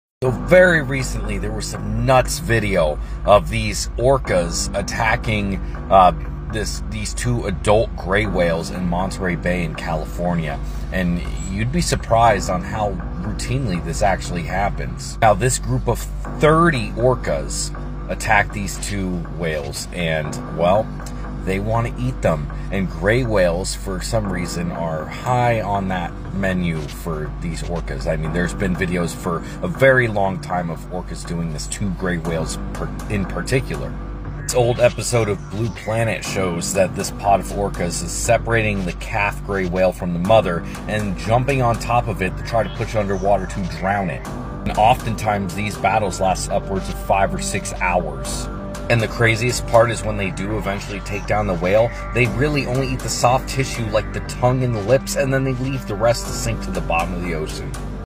Orcas love eating whale tongues
Orcas love eating whale tongue